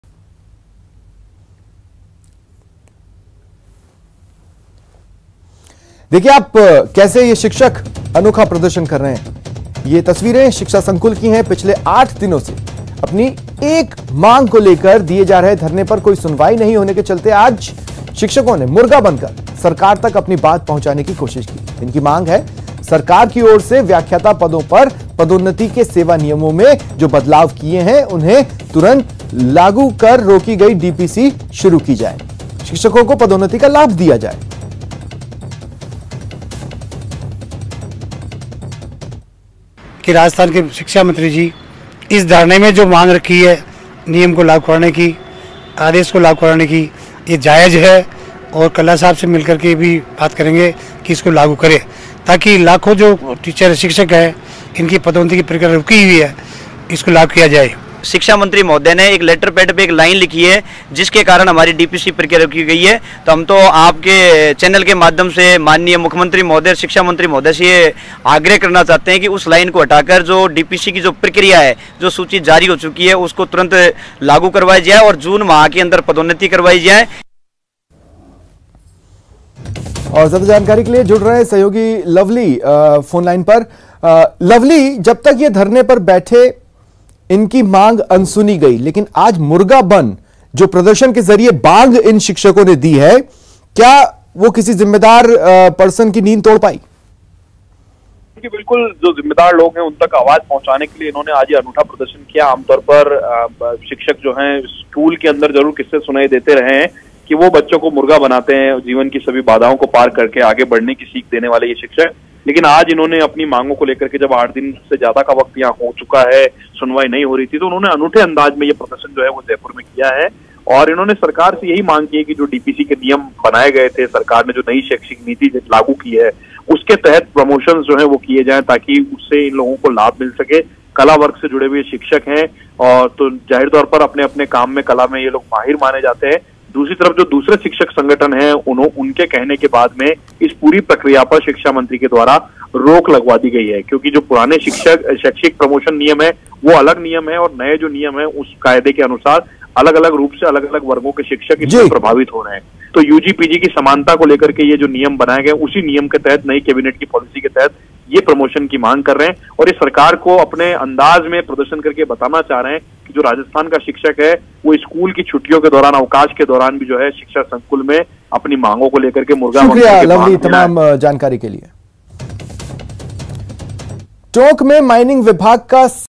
जानिए जब शिक्षक ही बन गए मुर्ग़ा Know why the teacher became a cock here
विद्यार्थियों को अपने जीवन में सभी बाधाओं से पार पाकर आगे बढ़ने की सिख देने वाले शिक्षक इस समय अपनी एक मांग को लेकर मुर्गा बनते हुए नजर आ रहे हैं,,जी राजस्थान,,,शिक्षा संकुल में कुछ ऐसा ही नजारा आज देखने को मिला,,जहां पिछले 8 दिनों से अपनी एक मांग को लेकर दिए जा रहे धरने पर कोई सुनवाई नहीं होने के चलते आज धरने में मौजूद शिक्षकों ने मुर्गा बनकर सरकार तक अपनी बात पहुंचाने की कोशिश की,,,,,,,शिक्षकों की मांग है की सरकार की ओर से व्याख्याता पदों पर पदोन्नति के सेवा नियमों में जो बदलाव किए हैं उनको तुरंत लागू कर रोकी गई डीपीसी को शुरू किया जाए साथ ही शिक्षकों को पदोन्नति का लाभ दिया जाए,,,

गौरतलब है कि शिक्षा विभाग की ओर से 3 अगस्त 2021 को शिक्षा सेवा नियमों में बदलाव करते हुए व्याख्याता पदों पर पदोन्नति के लिए यूजी और पीजी समान विषय से होने की अनिवार्यता लागू की गई,,जिसके विरोध में प्रदेश में शिक्षकों ने एक आंदोलन की शुरूआत की,,,आंदोलन शुरू होने के बाद शिक्षा विभाग की ओर से साल 2021-22 और 2022-23 की डीपीसी पर रोक लगा दी,,रोक के बाद कला वर्ग के शिक्षकों ने भी आंदोलन का रास्ता अपनाते हुए संशोधित नियमों के साथ ही डीपीसी कर पदोन्नति का लाभ देने की मांग को लेकर धरना शुरू किया,,,,

शिक्षकों द्वारा दिए जा रहे इस धरने को आज एससी,एसटी,ओबीसी अधिकारी कर्मचारी महासंघ का समर्थन मिला,,महासंघ अध्यक्ष रामस्वरूप मीणा ने आज धरना स्थल पहुंचकर अपना समर्थन दिया,,इस दौरान रामस्वरूप मीणा ने बताया कि "सरकार द्वारा कैबिनेट में लिया गया फैसला बिल्कुल सही  है,,,व्याख्याता पदों पर पदोन्नति लिए यूजी और पीजी समान विषय की जो अनिवार्यता लागू की गई है वो गुणवत्तापूर्ण शिक्षा के लिहाज से काफी फायदेमंद है,,ऐसे में जल्द से जल्द नियमों को लागू करते हुए शिक्षकों को पदोन्नति का लाभ देना चाहिए,,,,,"